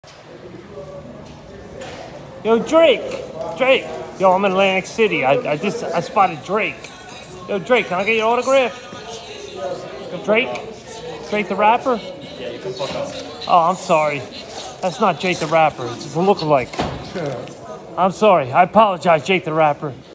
SPOTTED DRAKE IN ATLANTIC CITY!!
DRAKE WAS SPOTTED IN ATLANTIC CITY AND THERE ARE MANY MORE!!!